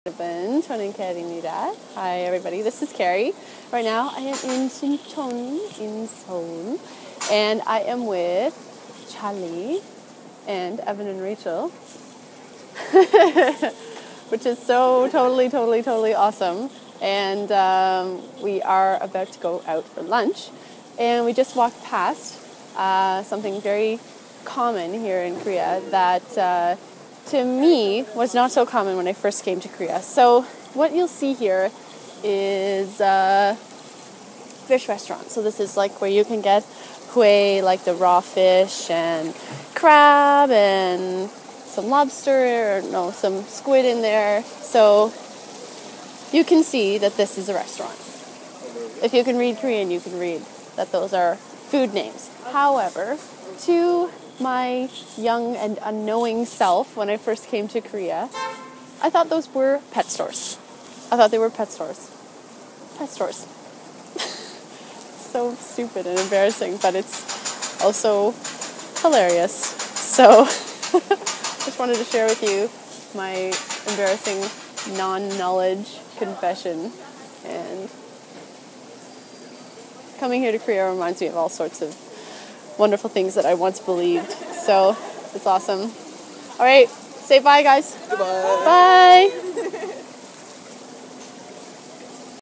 Expatkerri in Seoul: Pet Store or Restaurant?
The truth about what I thought these fishies were doing outside these businesses.